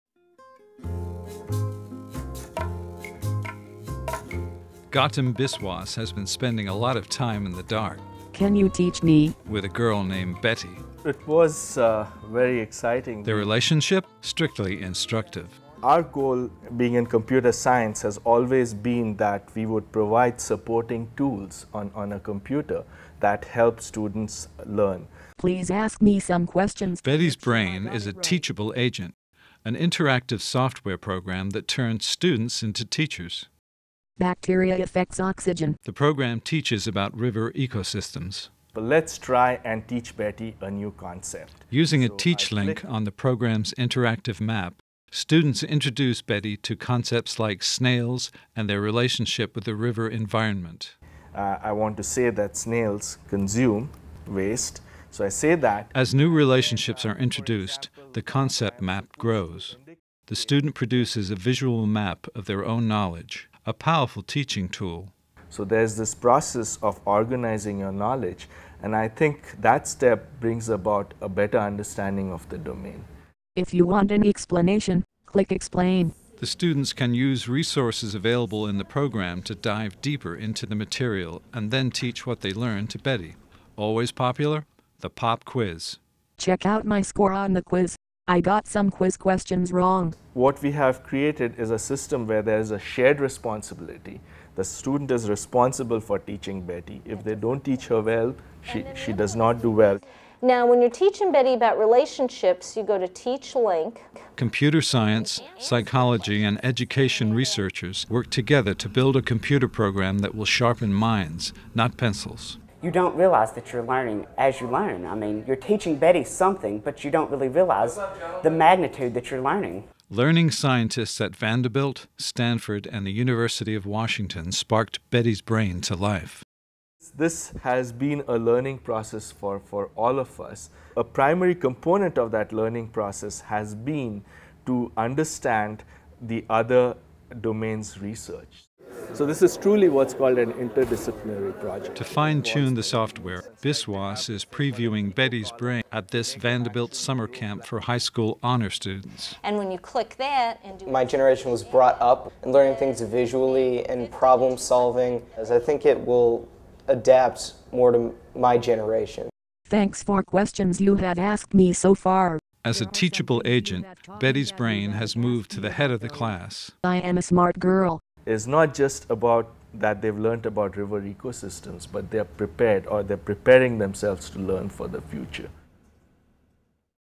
Betty's Brain - Conversational Agents in Science Learning
This video highlights the work of Dr. Gautam Biswas' in the construction of computer-based "conversational agents" to help children learn scientific concepts and check for understanding.